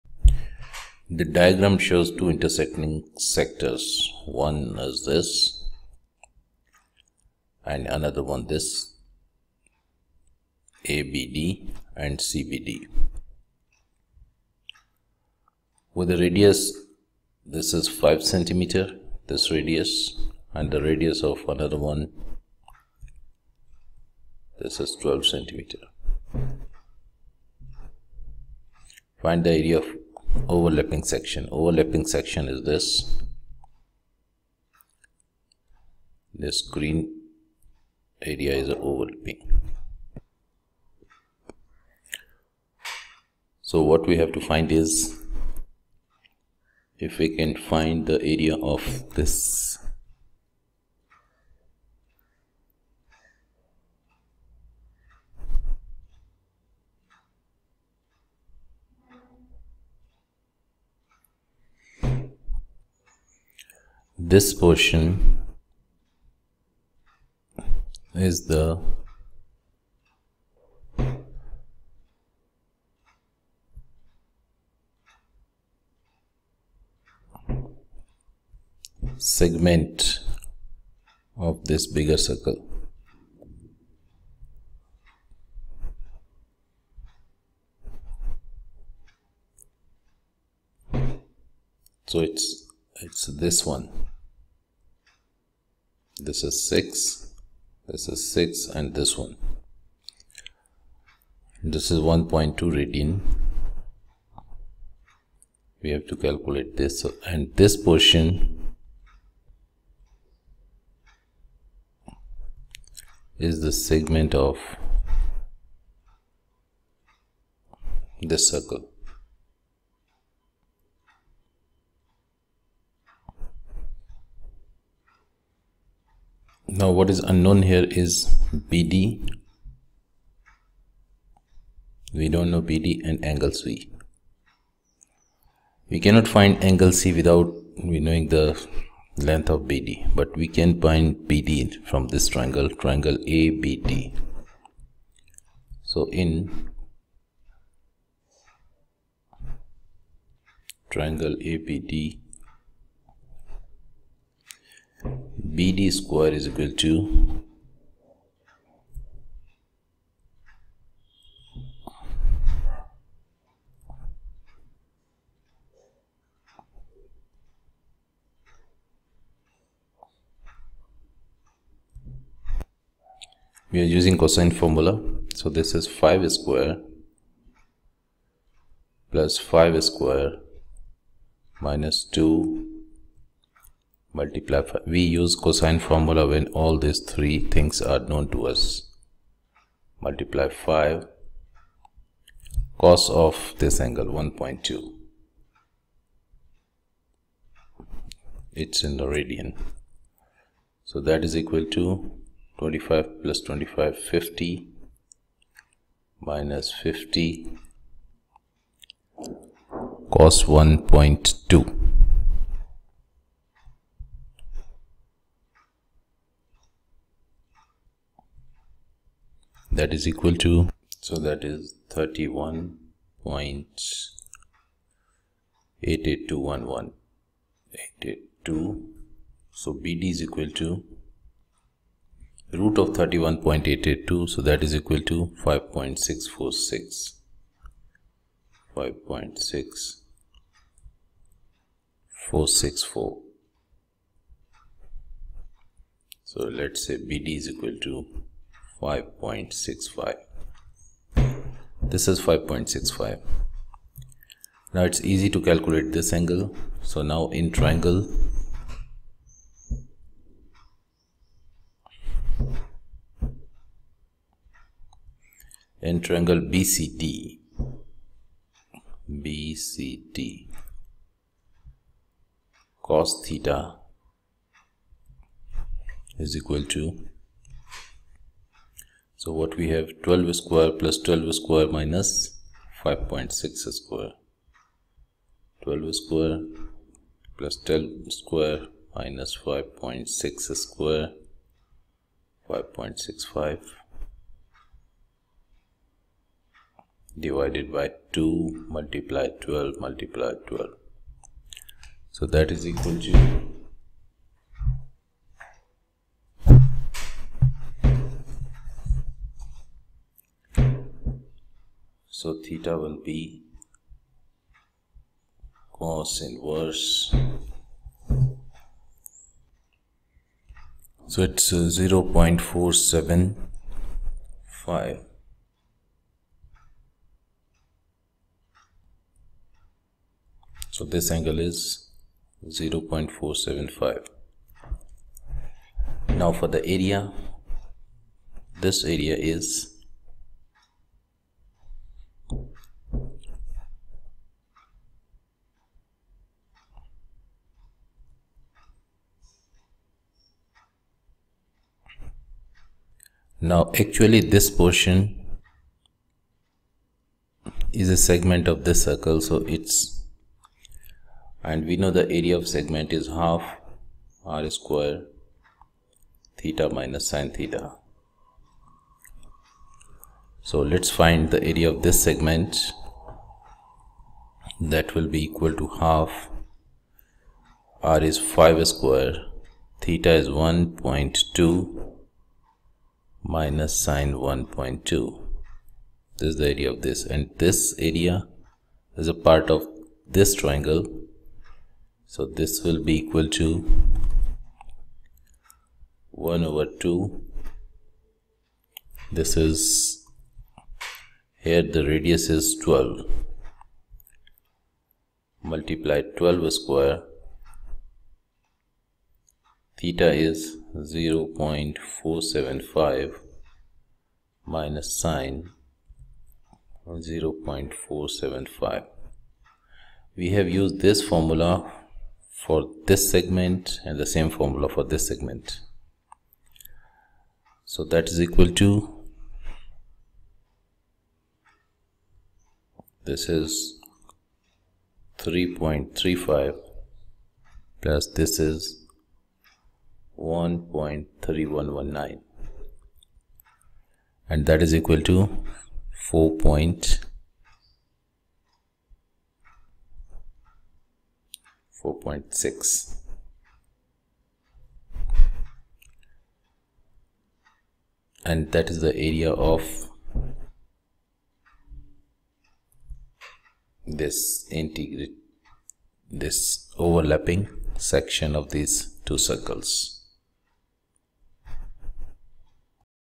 AREA OF OVERLAPPING CIRCLES
How to find the common area of two intersecting circles using Trigonometry.